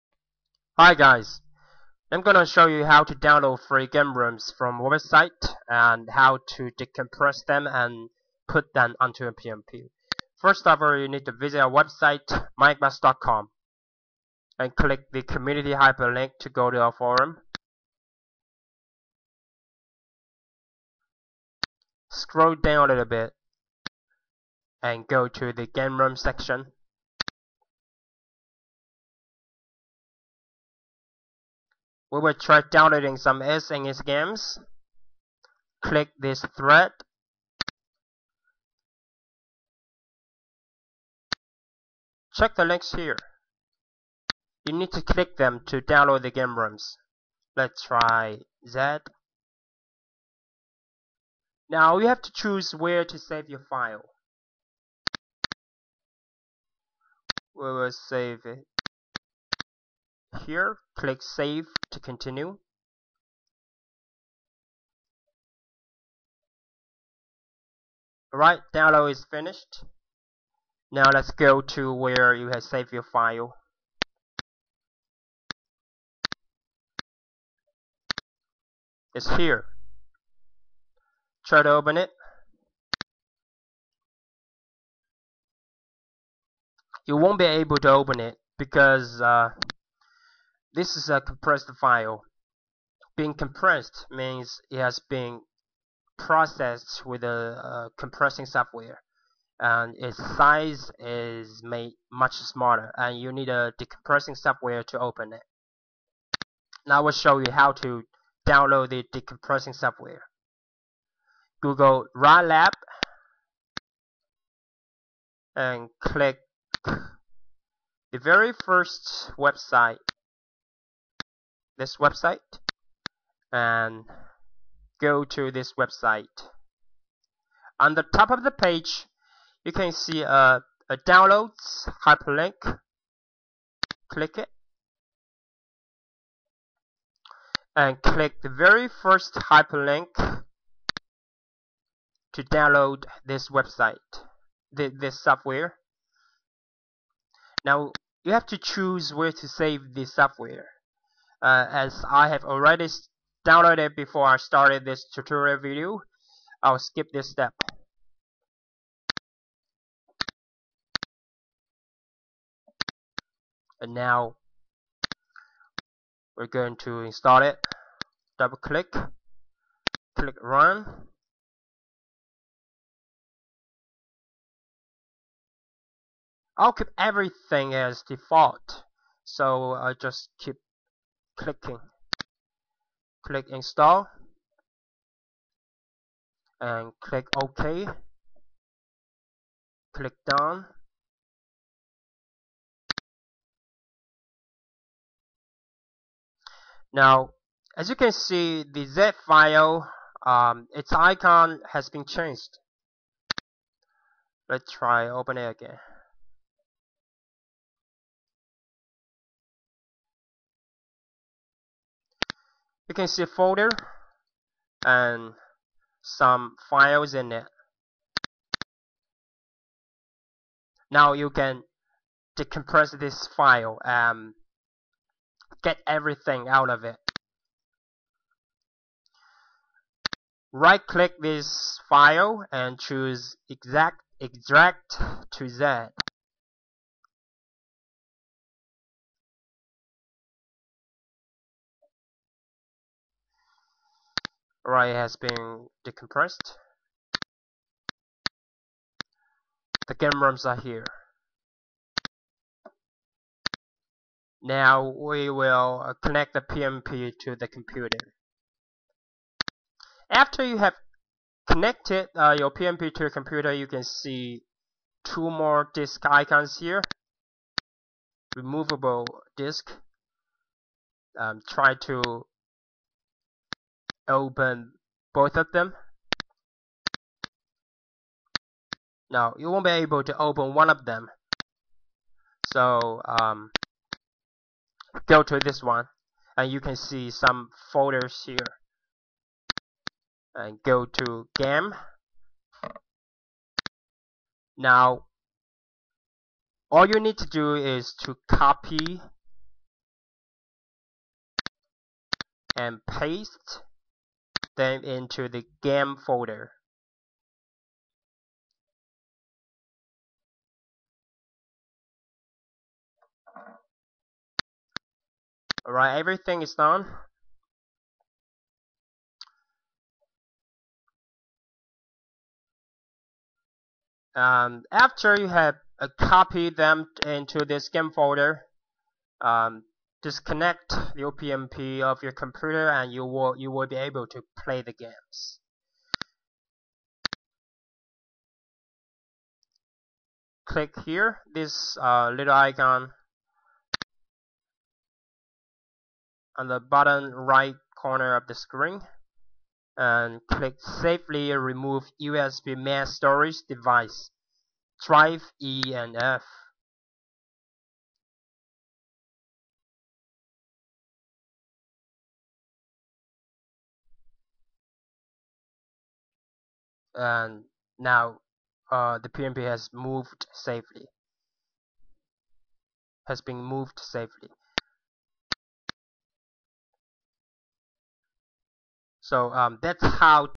How to download free SNES games roms and decompress / extract them
This video shows you how to download free game roms off our site. Suitable for not so tech-savvy people.